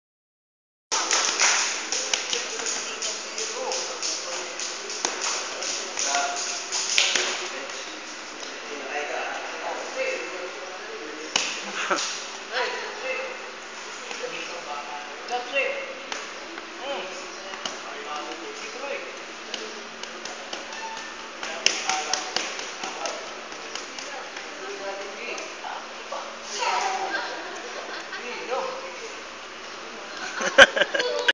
The Little Boxer of Tiger City Boxing Gym
Tiger city head trainer Tex Gonzaga's little boxer. Hey, coach, you have a slugger in the making.